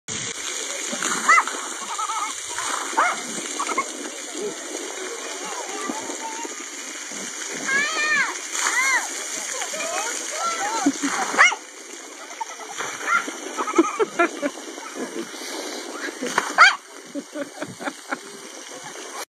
This is probably the happiest corgi in the world! Just needs water
In this cute dog and baby video, tune in to see some precious and hilarious moments between dogs, puppies, babies, and toddlers. Precious.